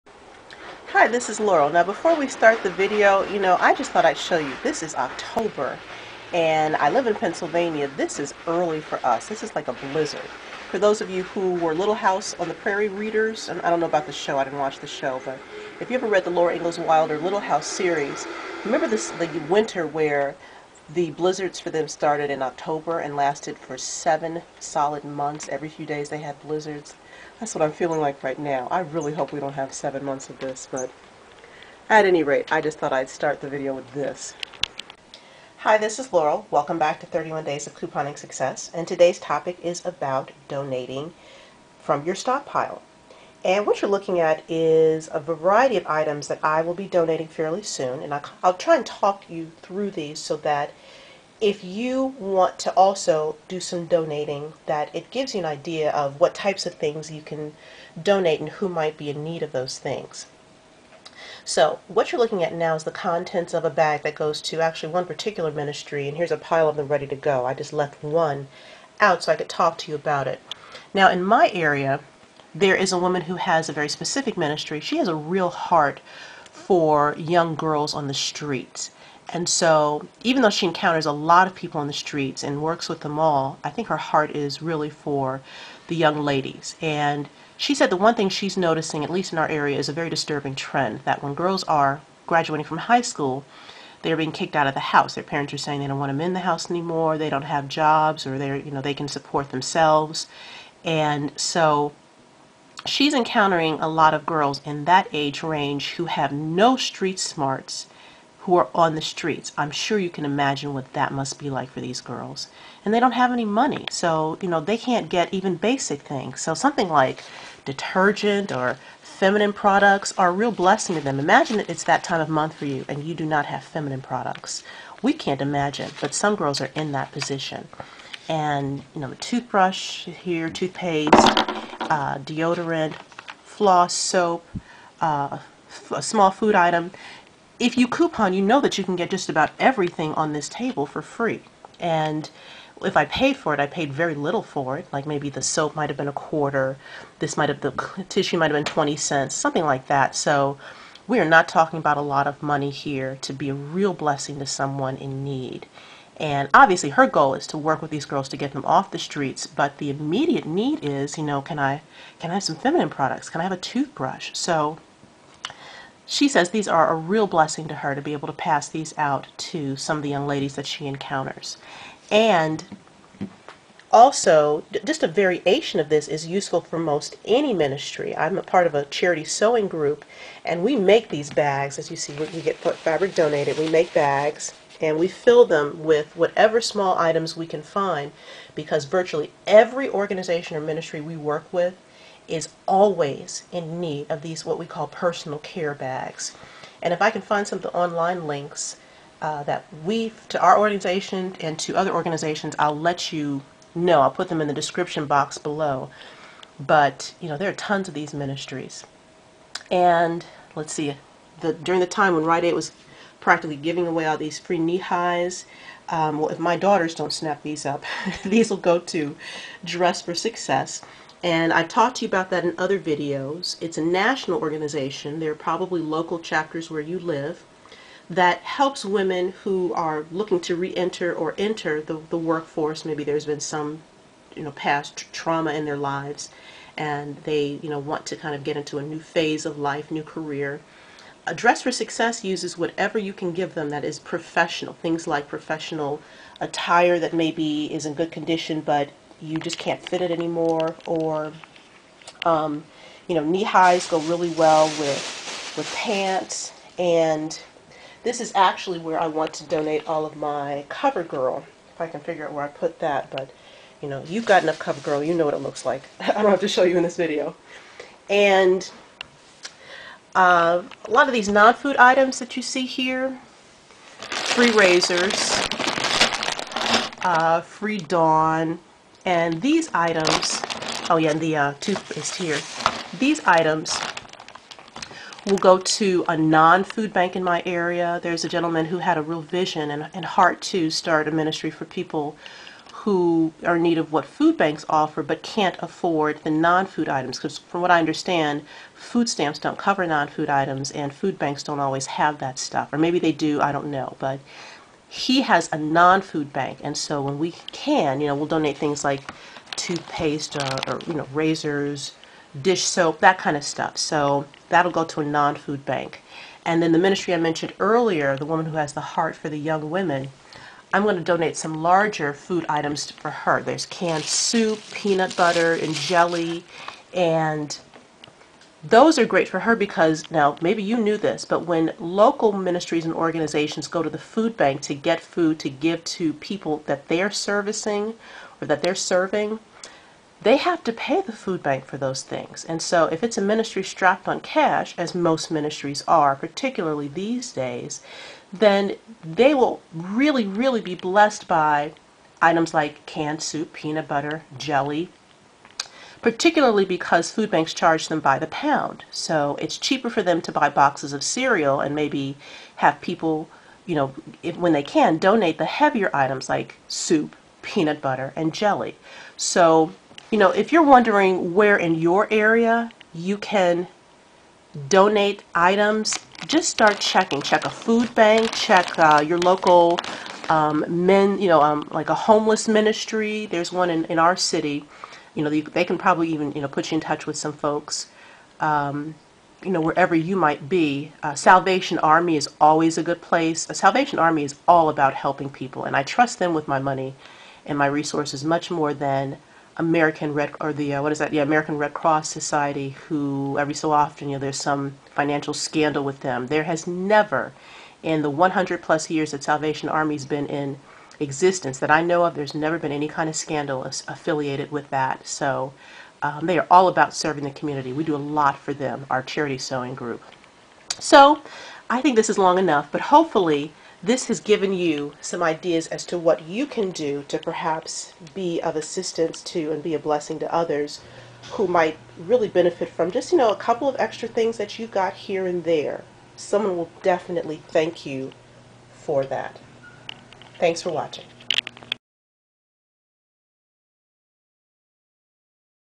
31 Days of Couponing! Day 29: Donating from your stockpile
Hopefully you have either begun to donate, or are seriously contemplating how you can donate from your stockpile. Food banks, and similar ministries and organizations around the country, are really hurting right now. Anything you have to give is greatly appreciated. This video presents some ideas.